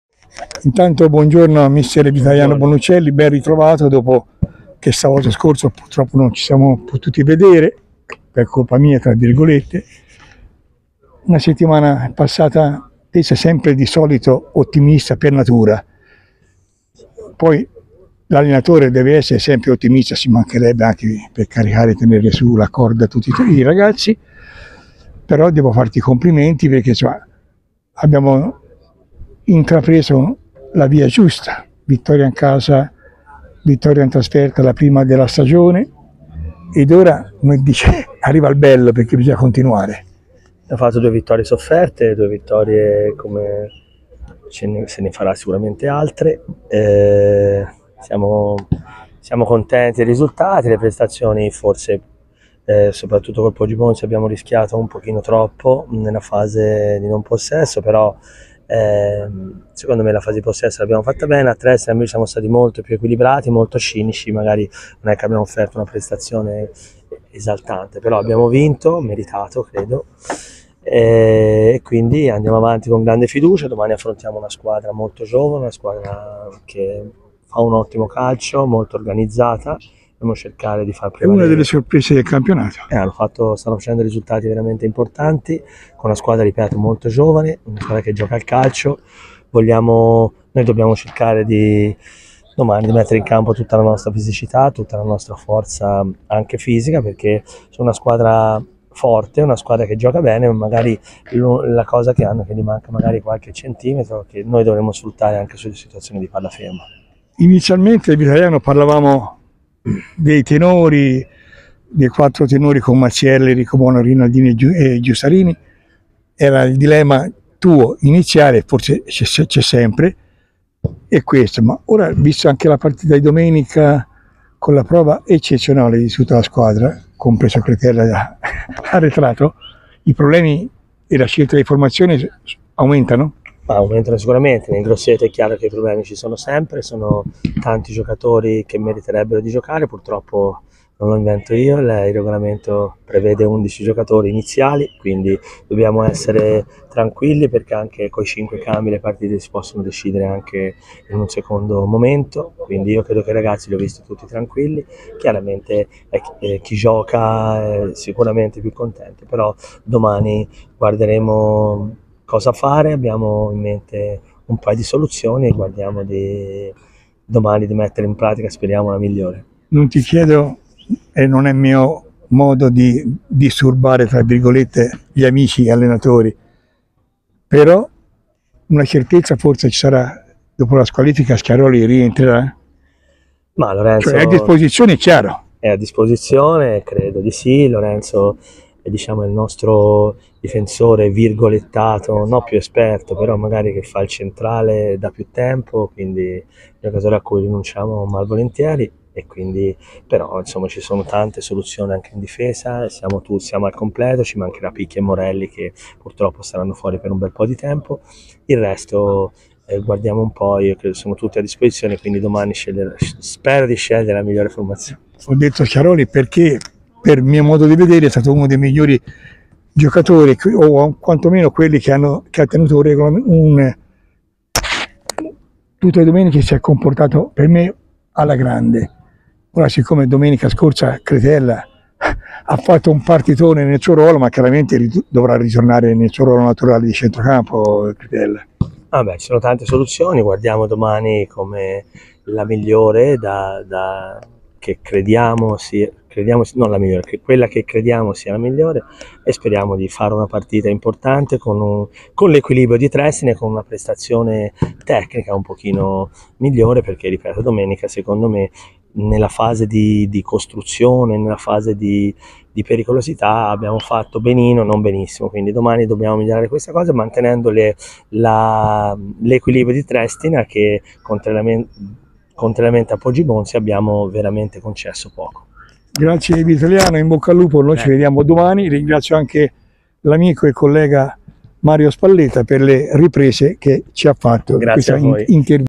GS TV - Intervista a Vitaliano Bonuccelli
Intervista al tecnico dell'Us Grosseto, Vitaliano Bonuccelli